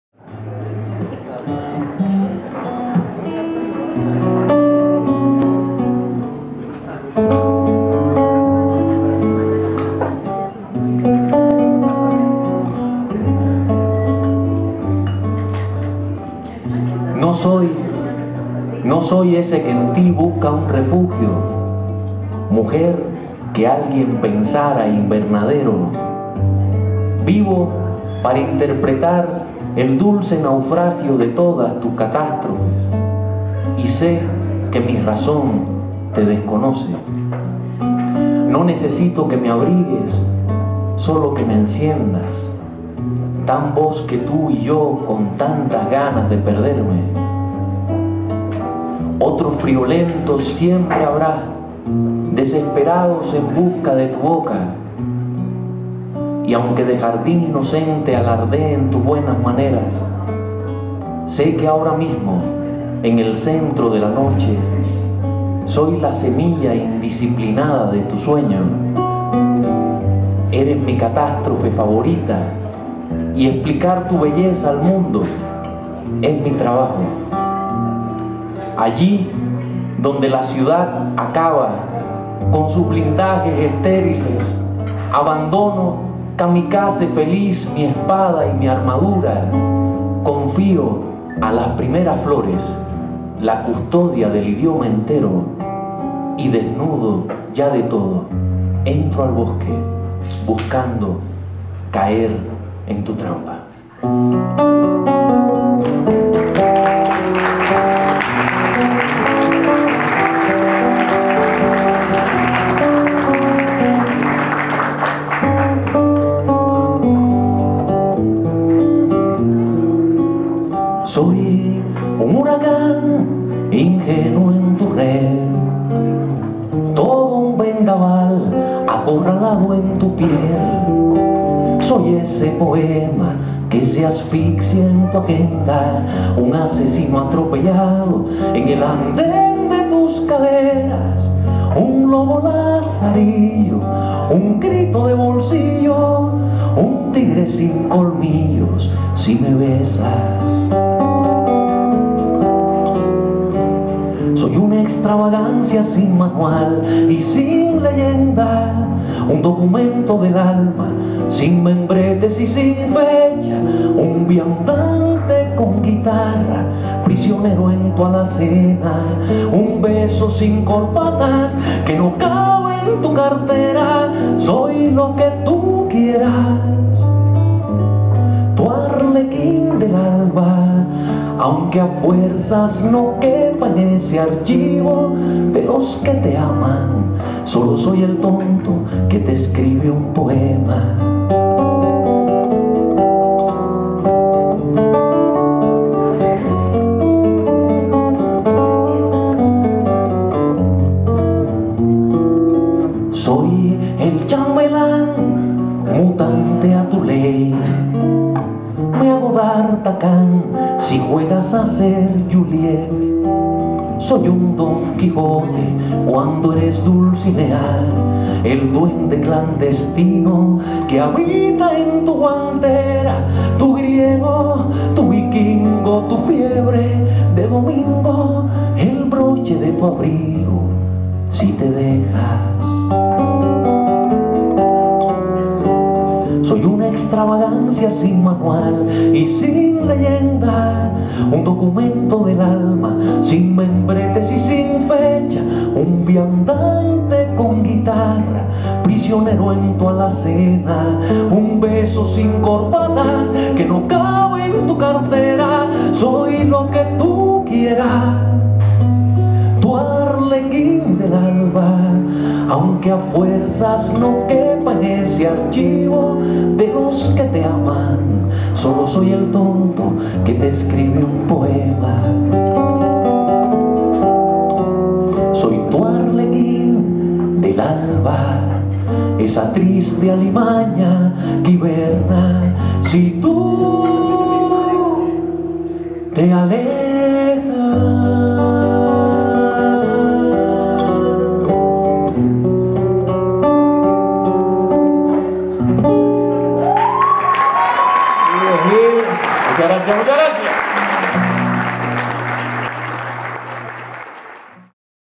POEMA QUE SE ASFIXIA - ADRIAN GIL TIGRE
Rodrigo Rojas y Adrian Gil Tigre
Teorema Puebla
17 de enero, 2013